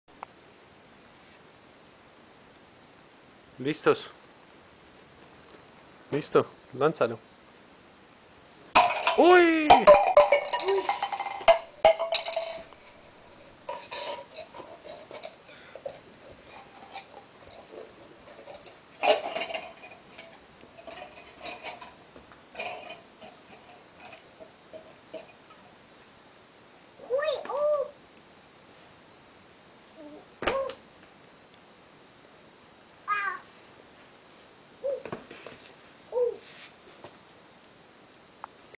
Angry Birds "Derrumbe de Vasos"
Unos Vasos de Angry Birds se destruyen ante la furia de un Angry Spiderman. Y algo de monerias.